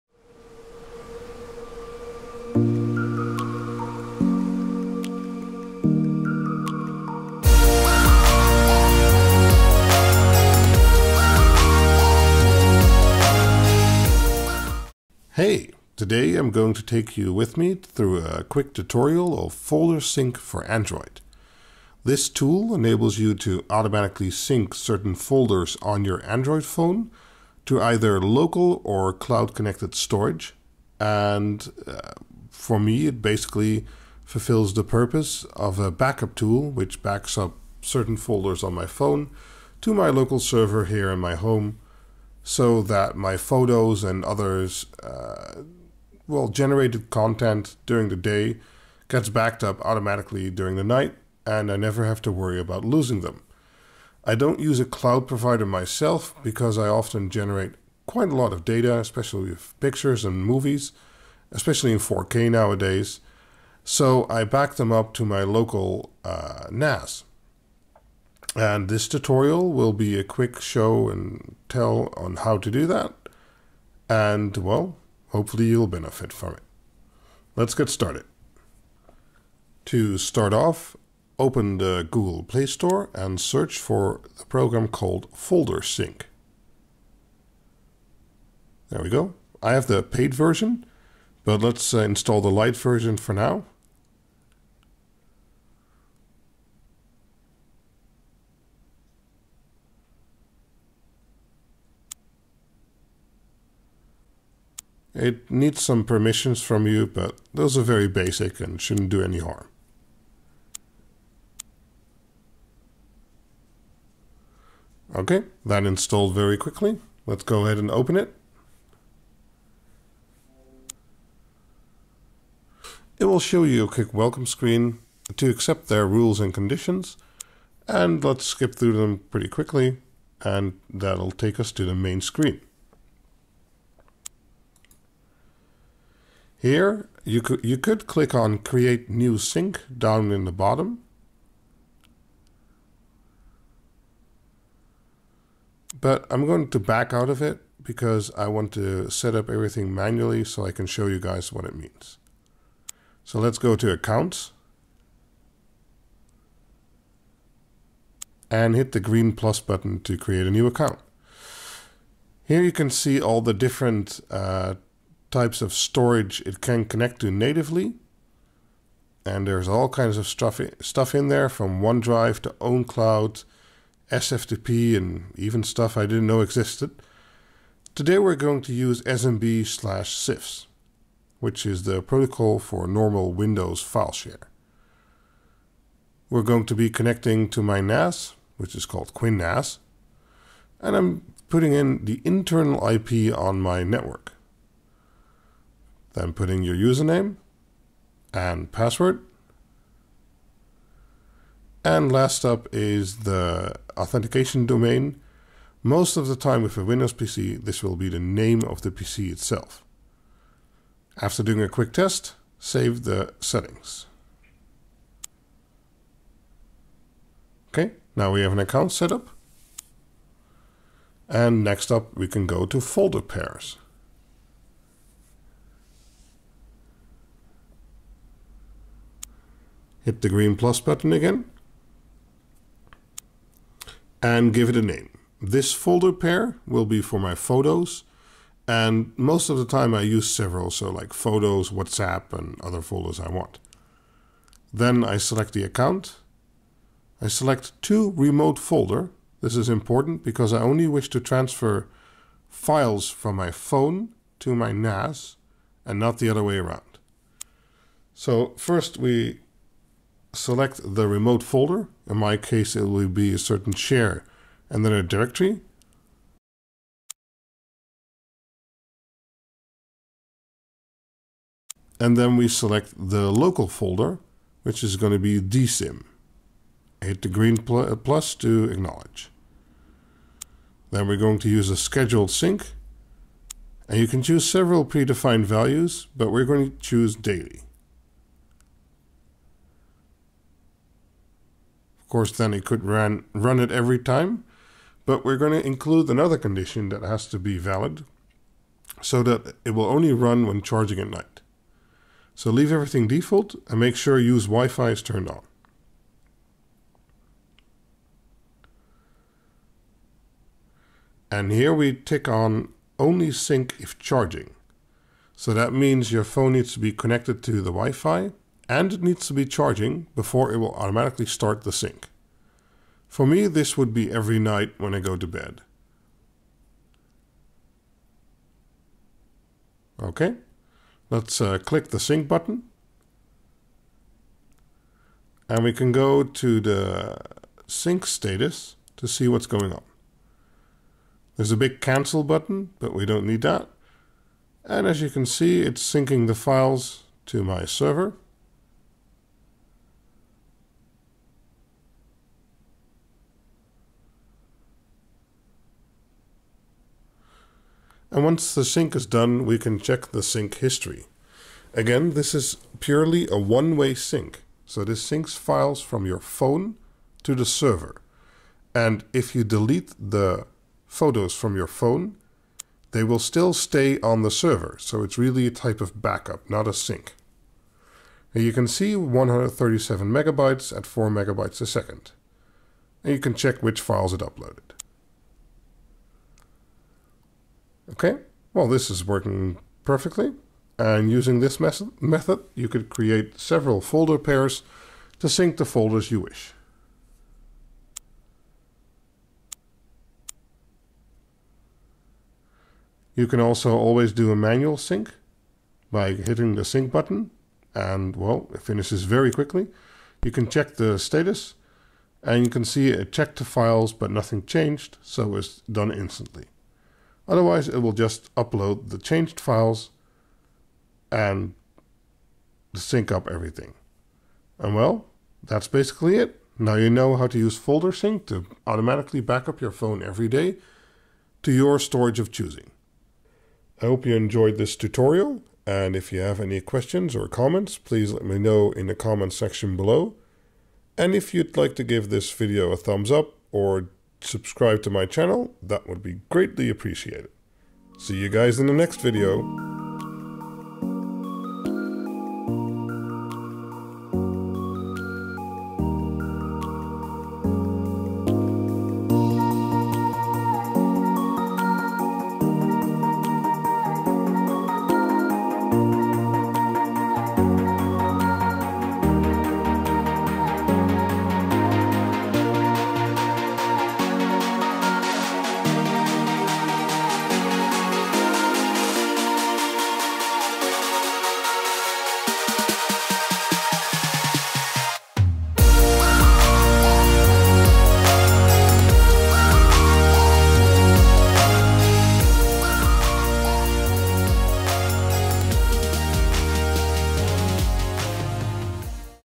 Tutorial: FolderSync
I use FolderSync to automatically backup several directories on my Android phone each night. I thought I'd make a tutorial so you can setup the same!